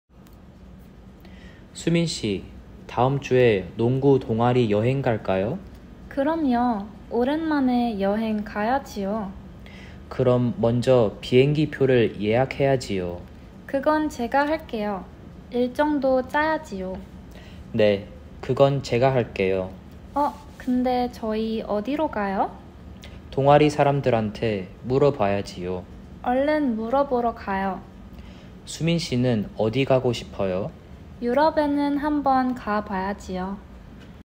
(K151) L5-5 (L4-5) 어야지요 2
Example 2:
다니엘: 수민 씨, 다음 주에 농구 동아리 여행 갈까요?
수민: 그럼요! 오랜만에 여행 가야지요.
다니엘: 그럼 먼저 비행기표를 예약해야지요.
수민: 그건 제가 할게요! 일정도 짜야지요.
다니엘: 네, 그건 제가 할게요.
수민: 어, 근데 저희 어디로 가요?
다니엘: 동아리 사람들한테 물어봐야지요.
수민: 얼른 물어보러 가요.
다니엘: 수민 씨는 어디 가고 싶어요?
수민: 유럽에는 한 번 가 봐야지요.

Example 2 (w English Translation):
Daniel: Soomin, how about going on a basketball club trip next week?
Soomin: Sure! It's been a while since we went on a trip.
Daniel: Then we should book the flight tickets first.
Soomin: I'll take care of that! We also need to plan the itinerary.
Daniel: Okay, I'll handle that.
Soomin: Oh, by the way, where should we go?
Daniel: We should ask the club members about that.
Soomin: Let's go ask them quickly.
Daniel: Soomin, where would you like to go?
Soomin: I want to visit Europe at least once.